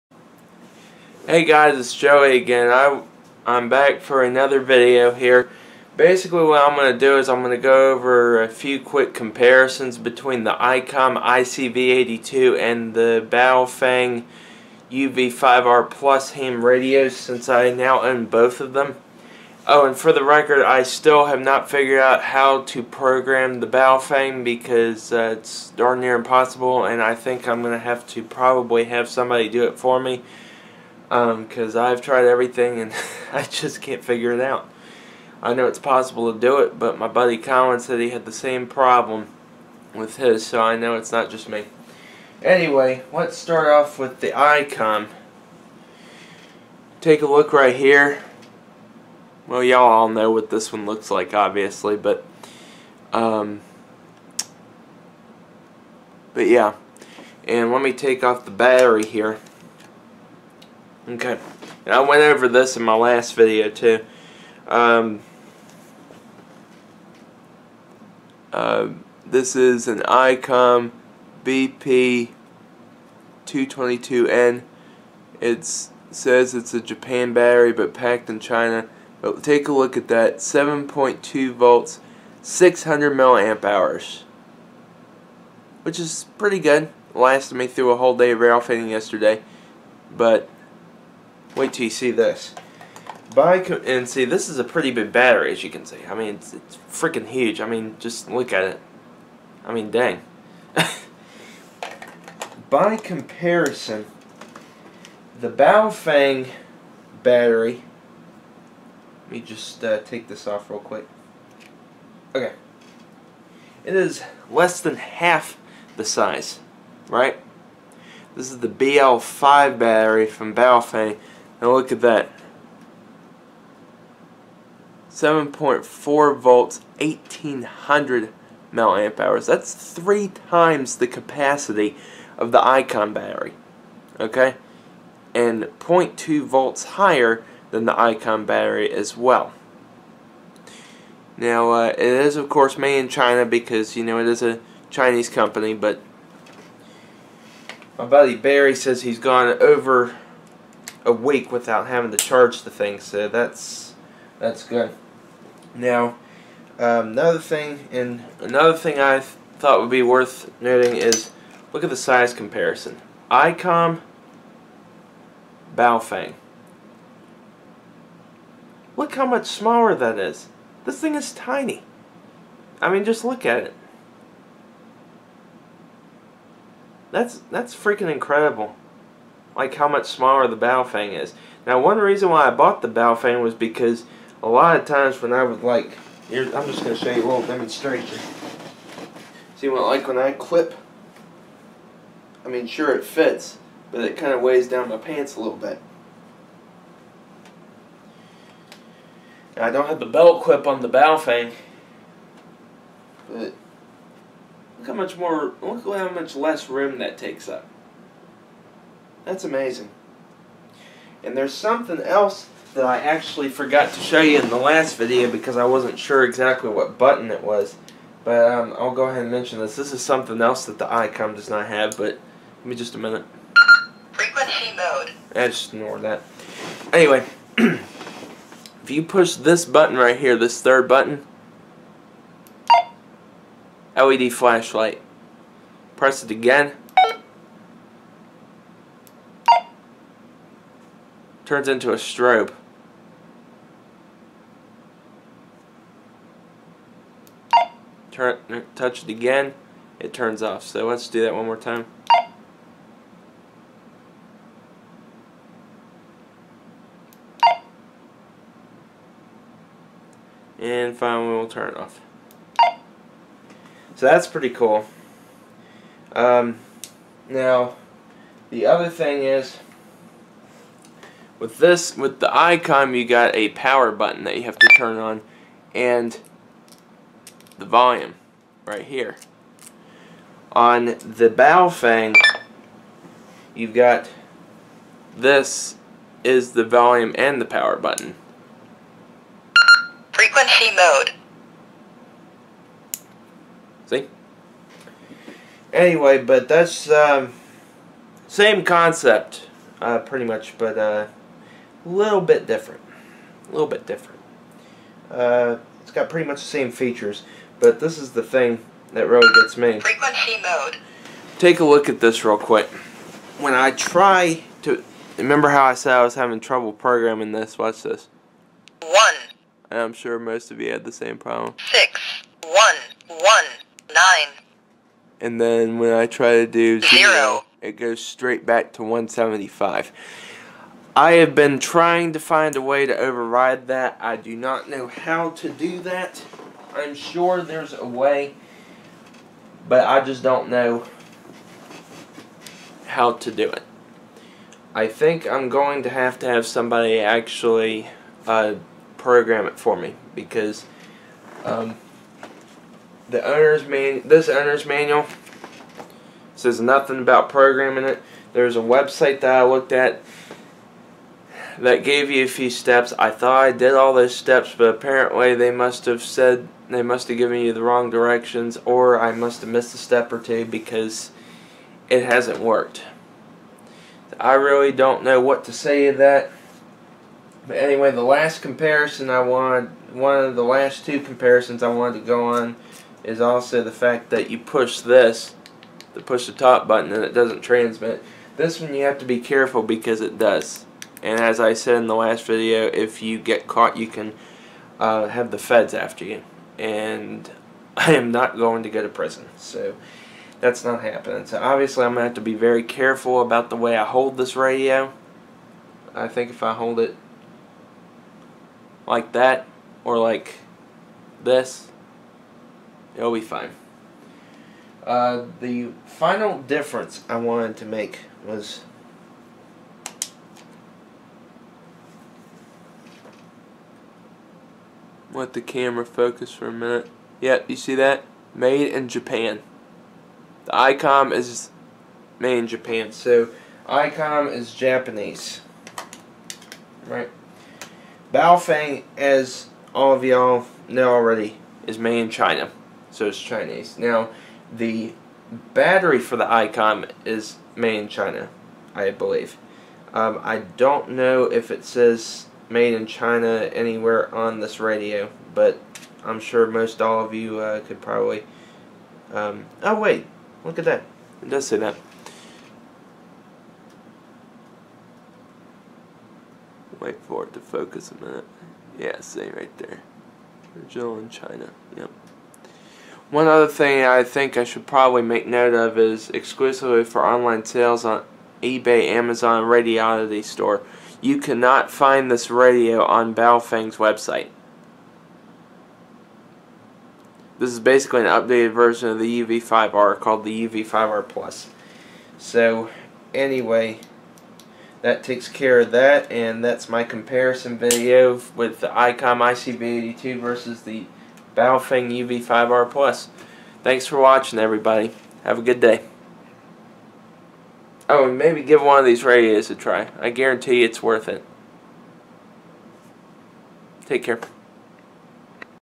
Baofeng UV-5R Plus vs. ICOM IC-V82
In this video, I go over differences and similarities between these two ham radios.

Regarding the ICOM:

-The ICOM is Japanese.
-This radio is $150, which is fifty dollars more.
-Only a 600mAh battery.
-In addition, the battery is Nickel-Cadmium (NiCd)
-No battery life indicator to tell how much life is left.
-Heavier and a little bulkier.
-Channels are locked, so you can't transmit on any channels without a license.
-BNC connector for antenna.

Regarding the Baofeng:

-The Baofeng is Chinese.
-This radio is $50 or less, over 50 dollars less than the Japanese counterpart.
-1800mAh battery, which is THREE times the capacity.
-In addition, the battery is a lithium-ion (Li-Ion)
-Battery indicator tells you how much life is left.
-Lighter and less bulky.
-Includes L.E.D. light/strobe.
-Channels are unlocked, so you can transmit on channels without a license. If you do that, especially on railroad channels, that can result in several federal charges.
-SMA connector for antenna.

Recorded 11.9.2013